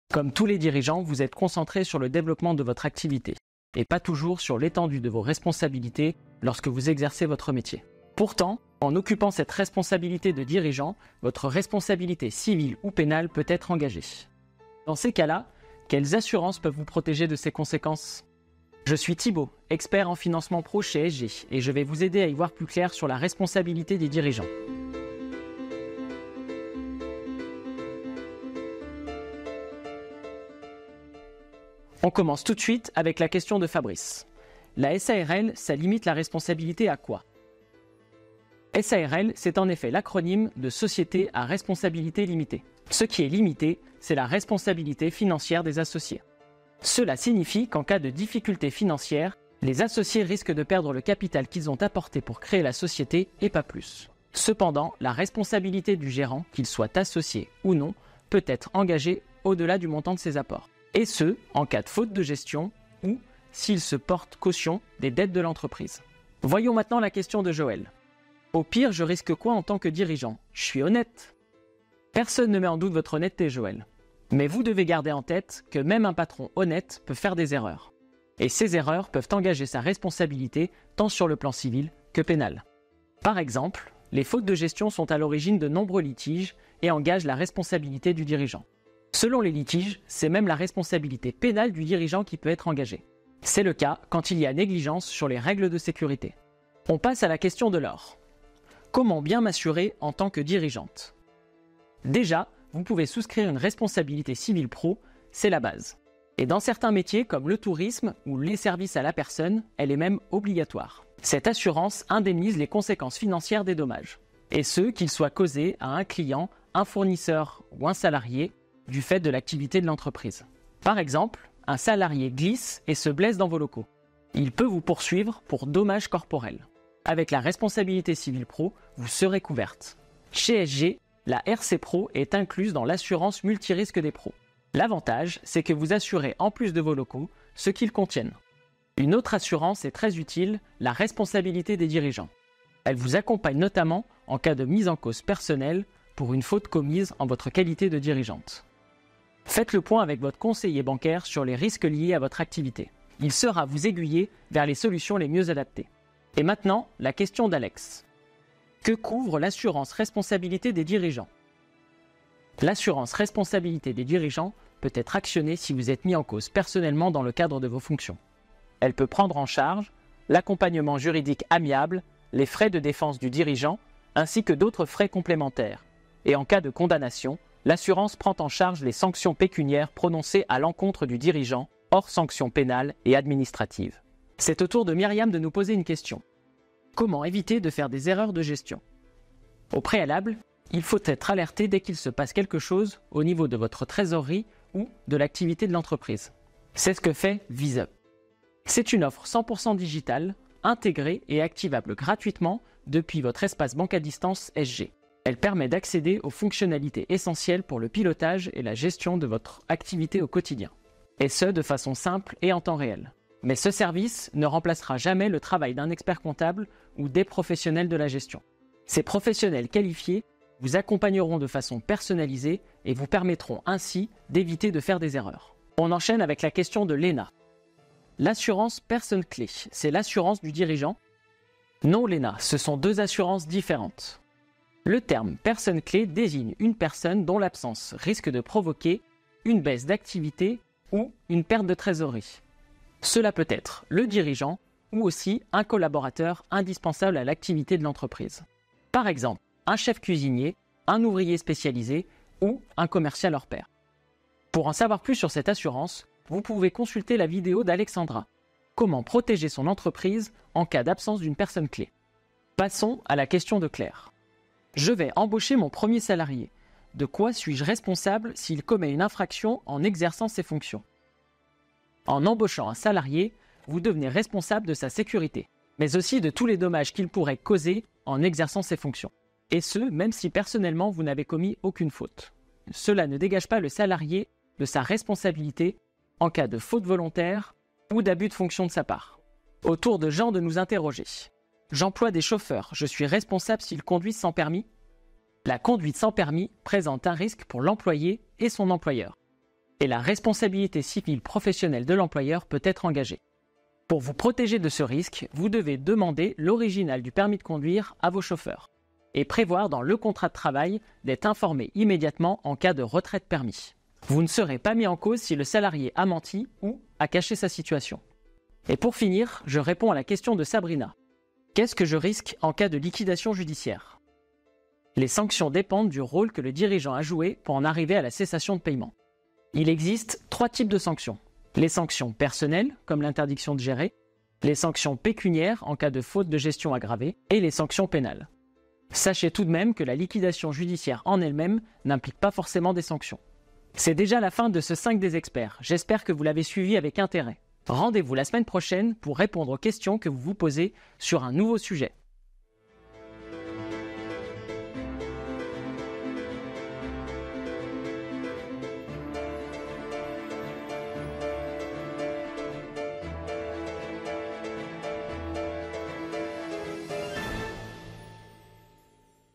La responsabilité des dirigeants | Le 5' des Experts - SG
Votre responsabilité civile ou pénale peut être engagée en cas d’infraction. Quelles sont les assurances pour vous protéger ? Notre expert répond en 5 minutes aux questions que vous pouvez vous posez.

00:00 Introduction
00:34 La SARL, ça limite la responsabilité à quoi ?
01:08 Au pire, je risque quoi, en tant que dirigeant ? Je suis honnête !
01:38 Comment bien m’assurer en tant que dirigeante ?
02:38 Que couvre l’assurance “Responsabilité des dirigeants” ?
03:06 Comment éviter de faire des erreurs de gestion ?
03:48 L’assurance Personne clé, c’est l’assurance du dirigeant ?
04:24 Je vais embaucher mon premier salarié. De quoi suis-je responsable s’il commet une infraction au travail ?
04:51  J’emploie des chauffeurs, je suis responsable s'ils conduisent sans permis ?
05:21  Qu’est-ce que je risque en cas de liquidation judiciaire ?
05:44 Conclusion